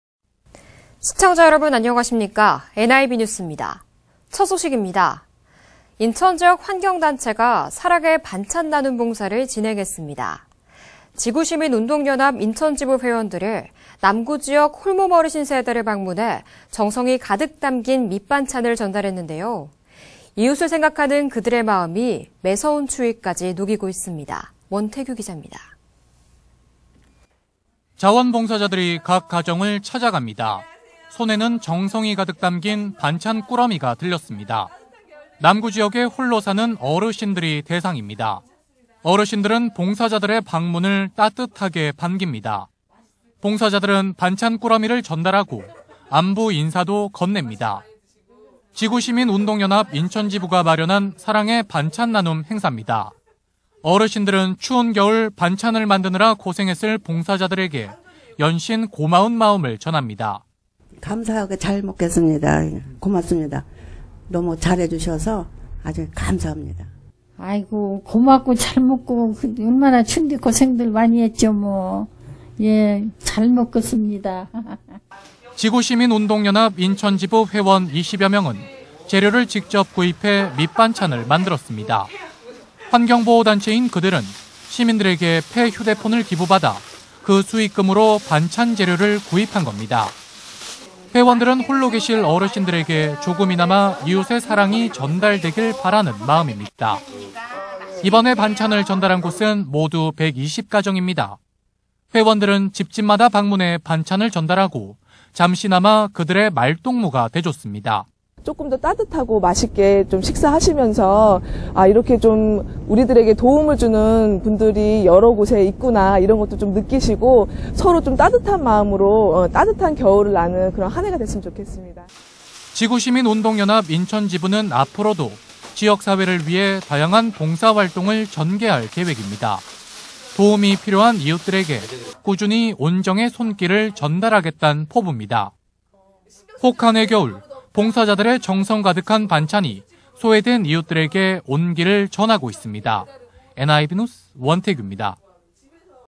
인천지구시민운동연합 - 20171226 남인천 방송
인천 지구시민회원분들이 기부해주신 중고 휴대폰 판매금으로 인천지역 홀몸어르신들께 사랑의 반찬배달을 하였습니다. 인천의 사랑나눔실천은 계속 됩니다~^^